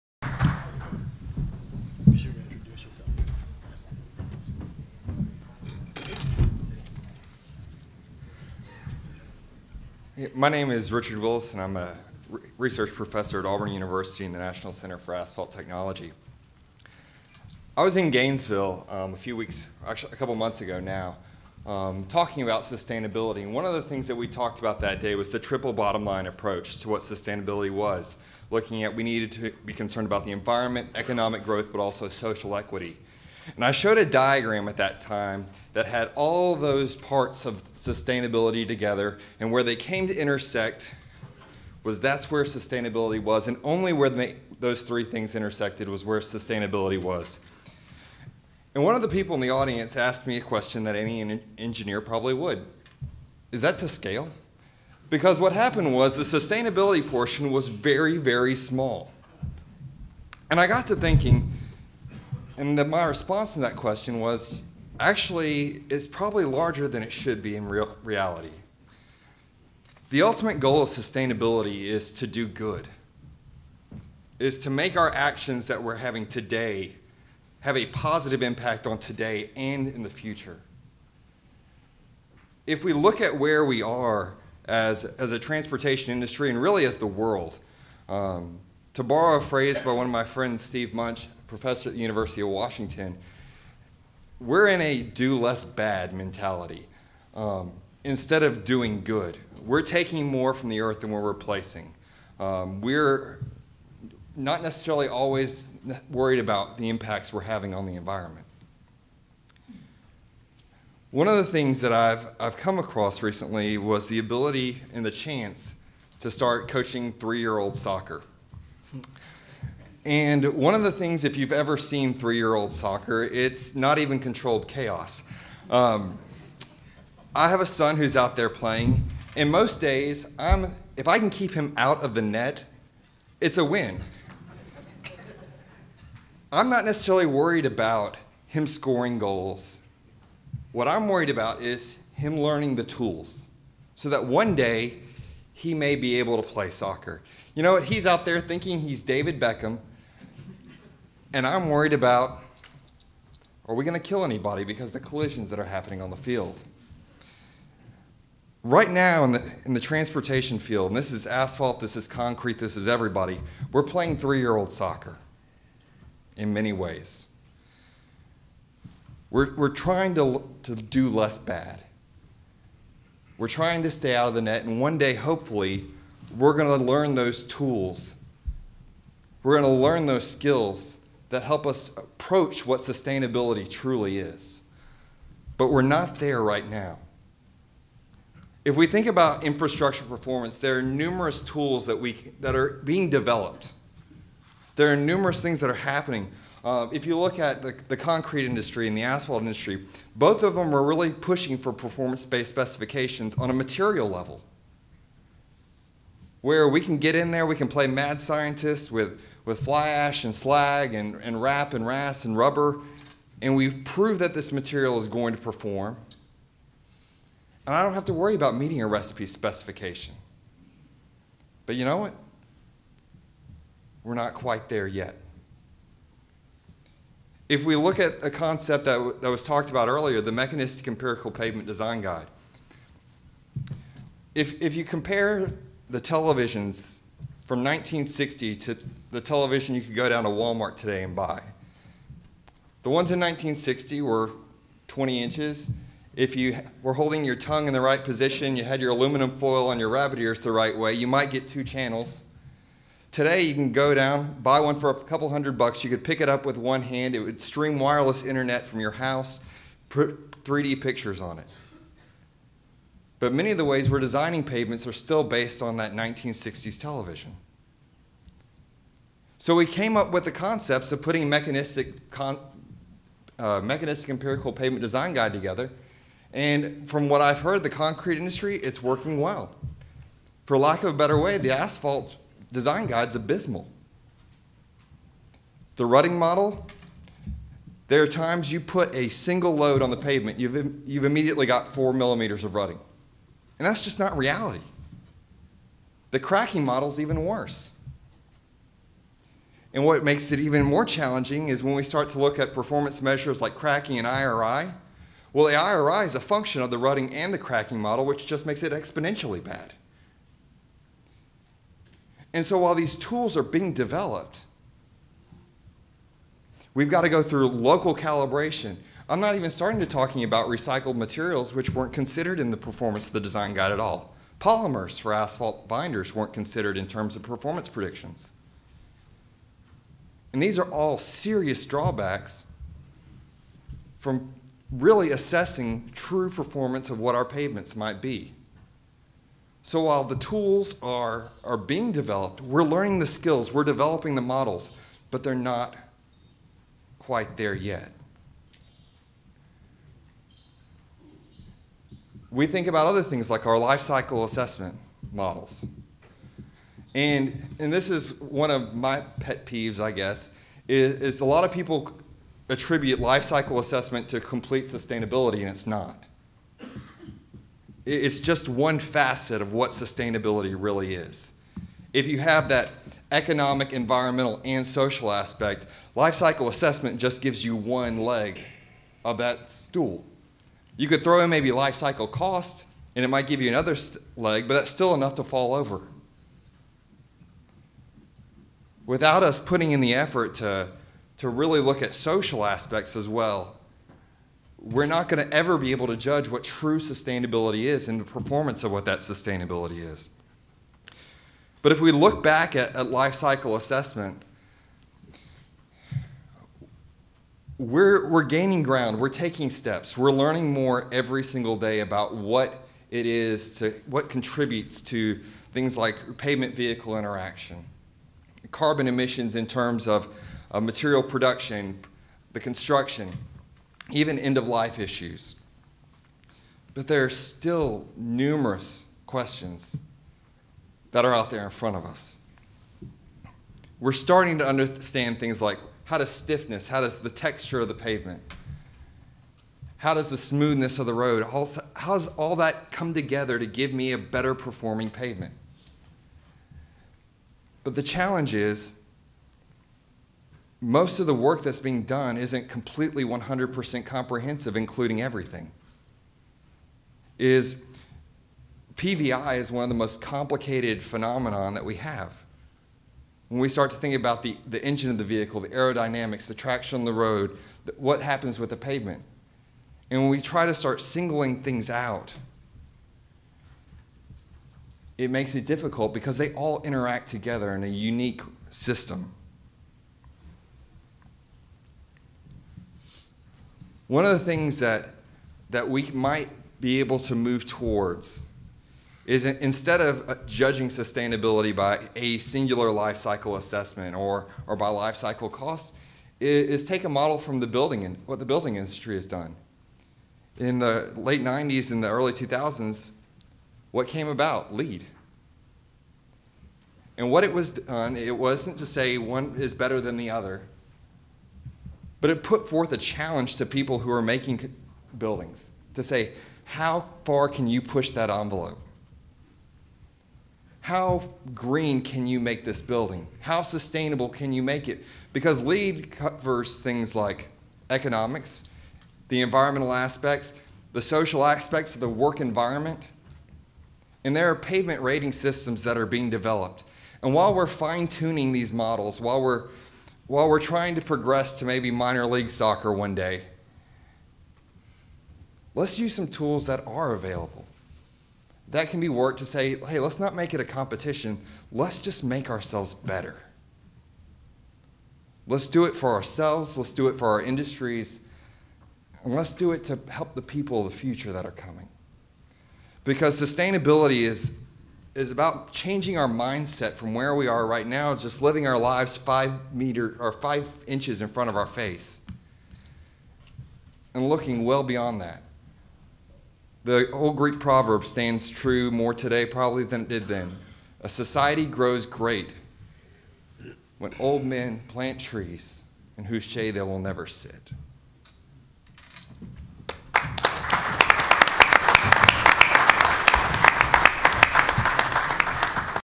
Panel 2, Richard Willis
Richard Willis is an Assistant Research Professor at Auburn University in the National Center for Asphalt Technology. He teaches Basic Construction Materials and Design and Assessment of Sustainable Pavements.